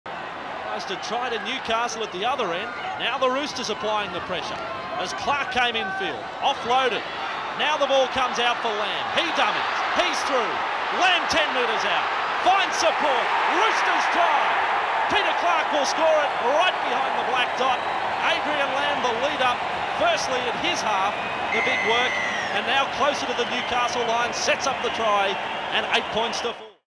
Adrian Lam sets up Peter Clarke (1995)
Roosters halfback Adrian Lam sets up a neat try for Peter Clarke in a 1995 clash with Newcastle.